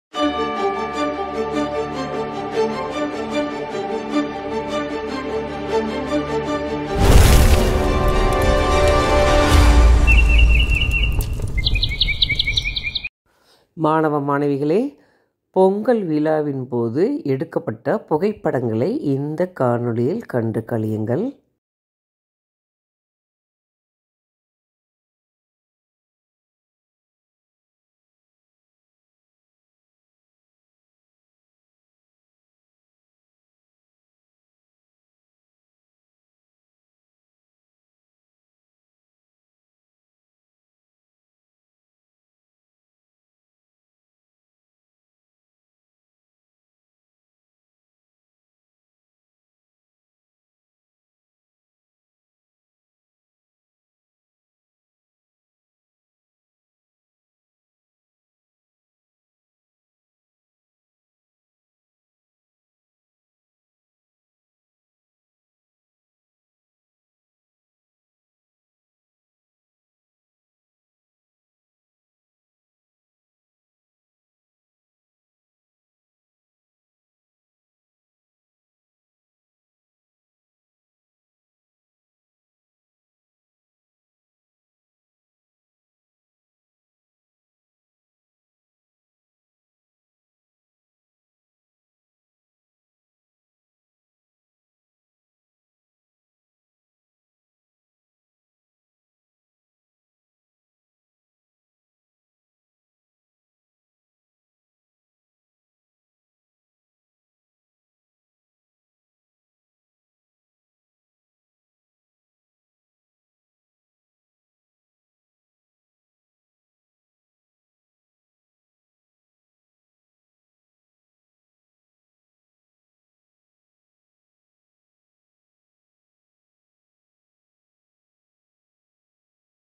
பொங்கல் விழாவின்போது எடுக்கப்பட்ட புகைப்படங்கள்:Pongal Festival Photos:VHSS.
Pongal Function Photos:
பொங்கல் விழாவின்போது எங்கள் பள்ளியில் எடுக்கப்பட்ட புகைப்படங்கள். Date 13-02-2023 Friday.
பொங்கல் விழா
பொங்கல் புகைப்படங்கள்
பொங்கல் கோலம்
சர்க்கரை பொங்கல்
Save Agriculture,விவசாயத்தை காப்போம்.